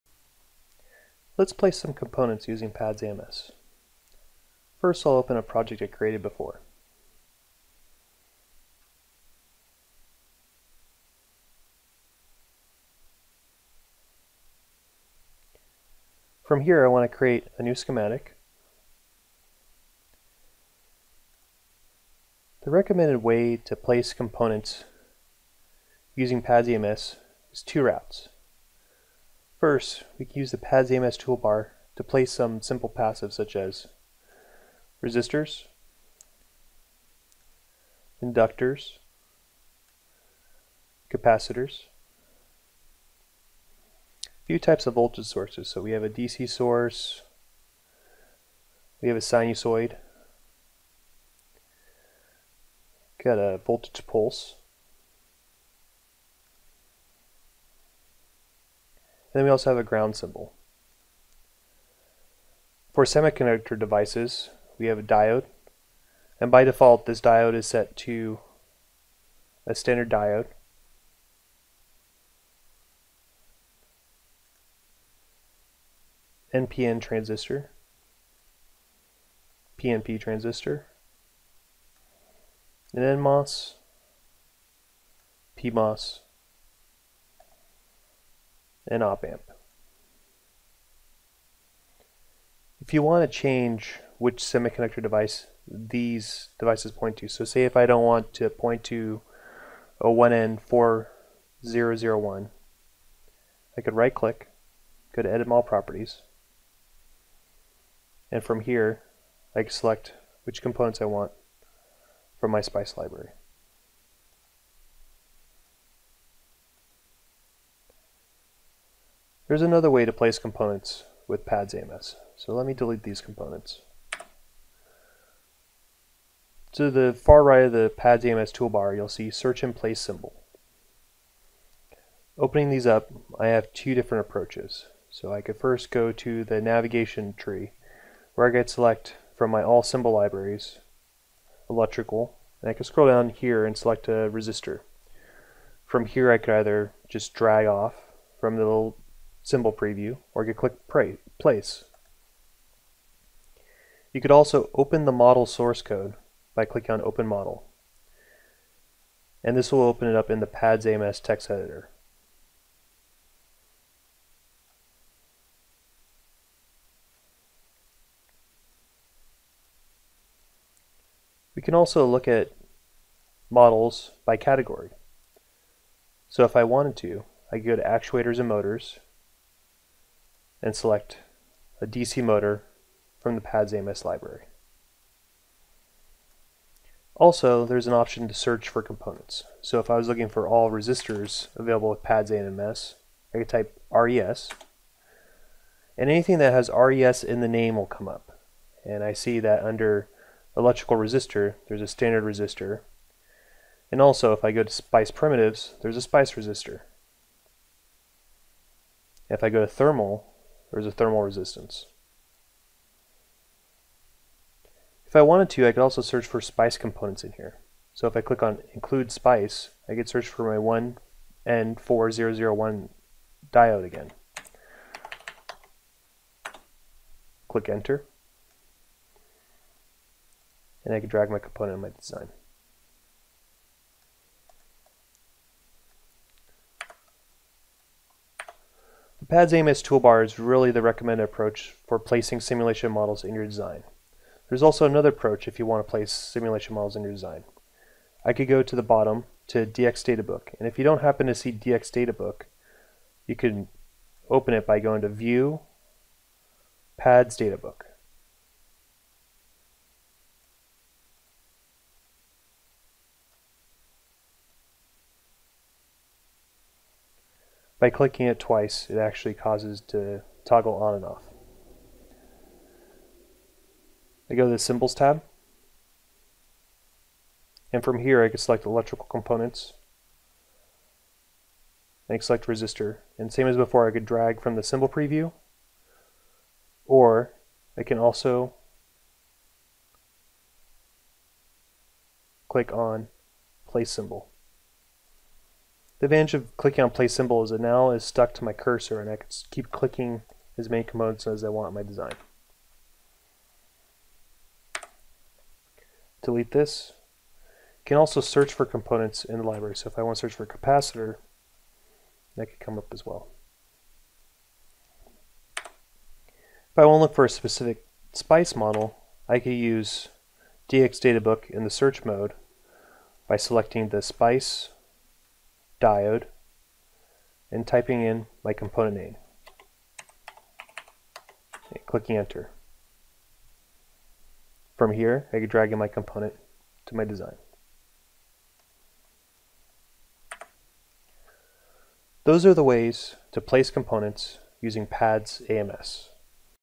Placing a Component using PADS AMS
How to place components using PADS AMS multi-physics simulation enviornment